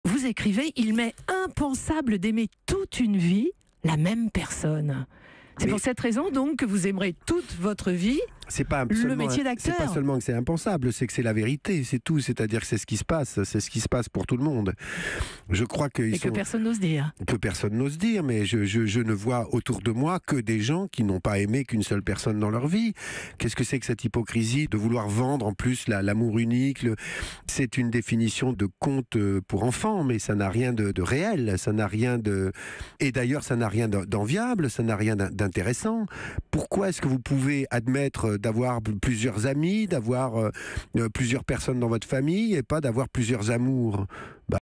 Charles Berling : "L'amour unique ? C’est un conte pour enfants"
A l'image du contenu de son dernier ouvrage  quot;Un homme sans identité quot;, le comédien a confié pensées et sentiments à Isabelle Morizet, au micro d'Europe 1.